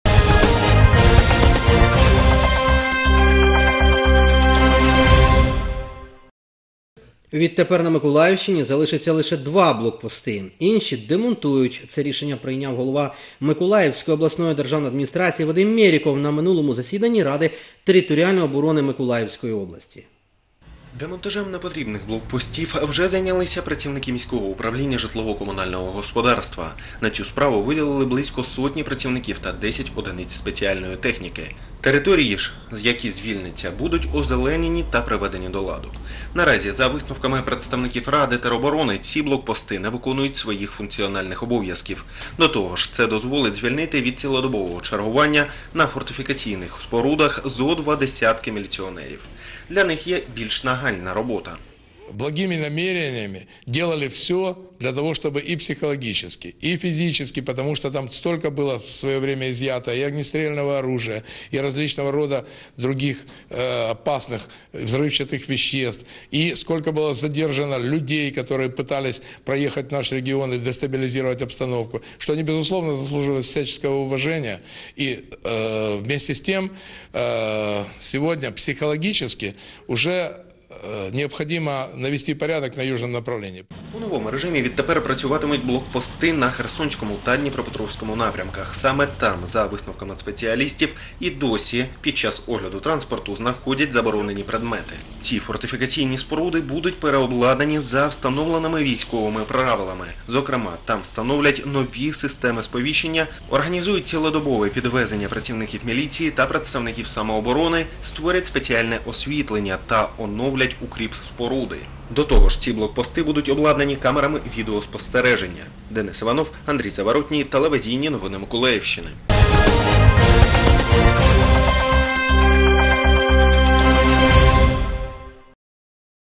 ТНМ сюжет 29.07.2015
На Миколаївщині залишаться два блокпости.
Автор - Денис Іванов.